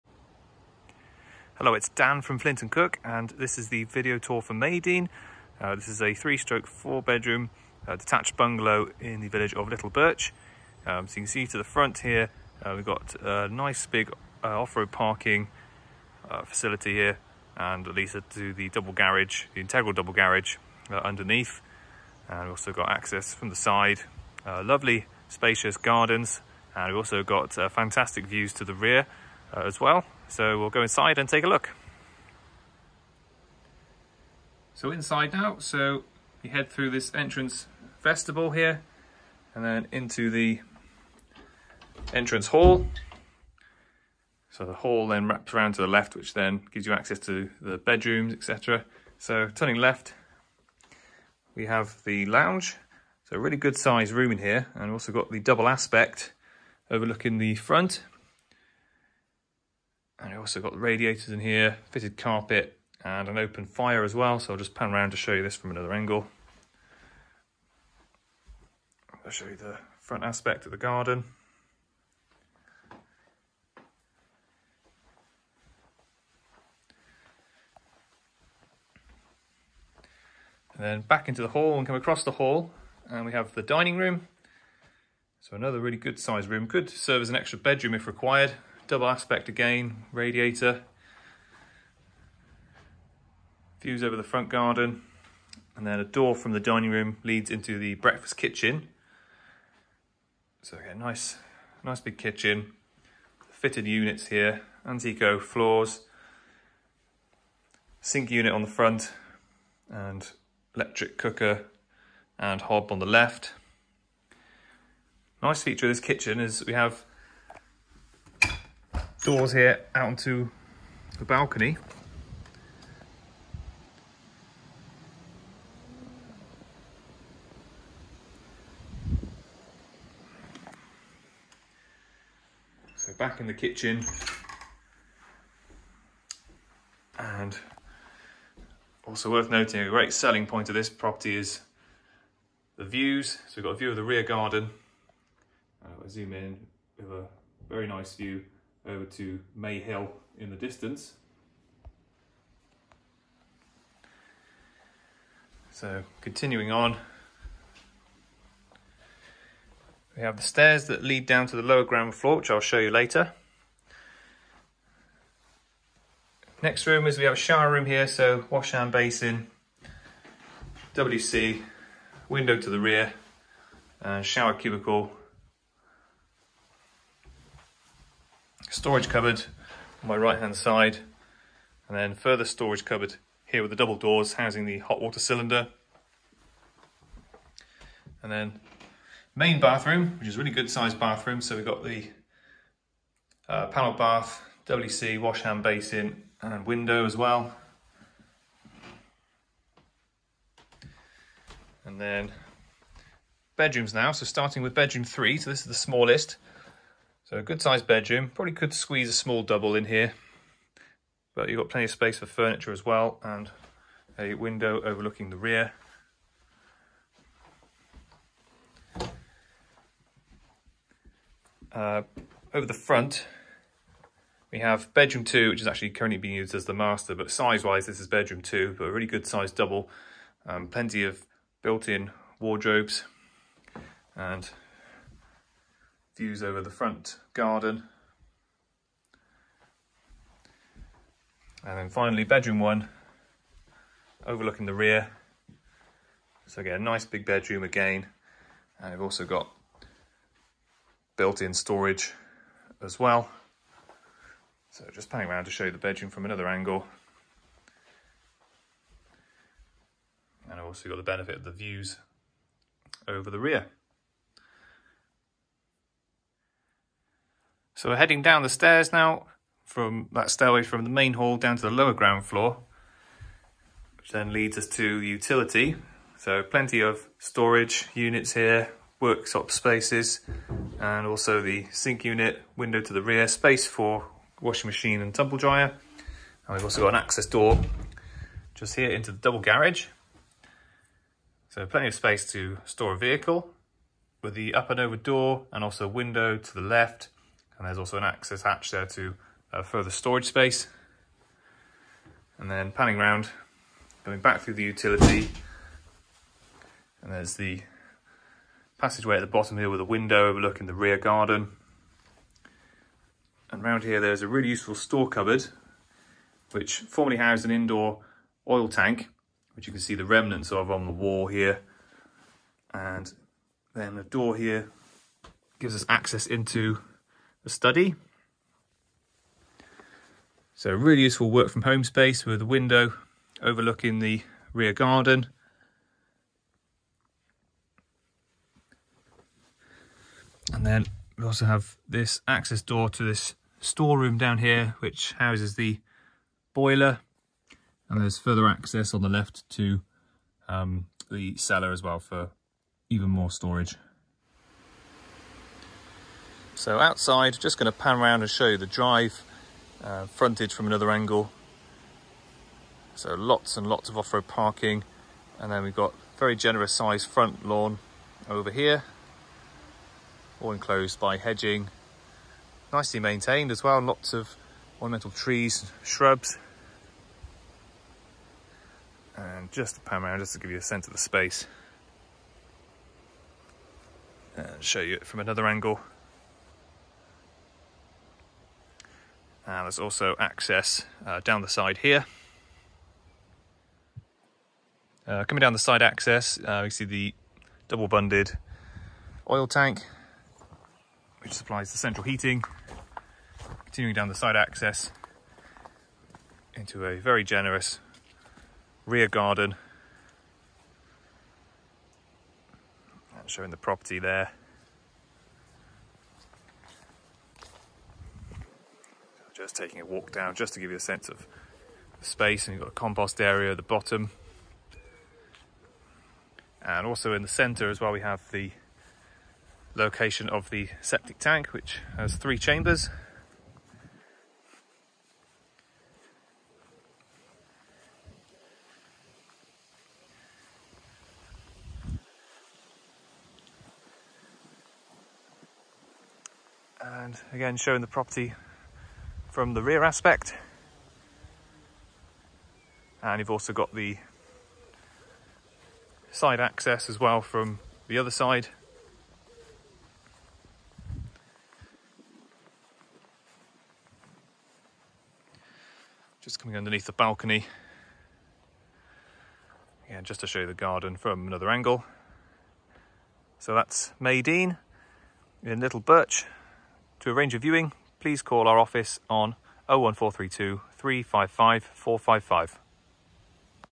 Video Tour - Little Birch Hereford
Within Little Birch/Kingsthorne villages there is an active community with a bus service, parish hall, church and public house. Also in nearby Much Birch there is a primary school and doctor's surgery.

The property was constructed in the mid-1970's and it is essentially a bungalow with lower ground floor and is ideal for family purposes having spacious accommodation (2200 sq.ft. including the garage). There is double-glazing (sealed units), oil-fired central heating, a good-sized garage and very large main, lawned garden.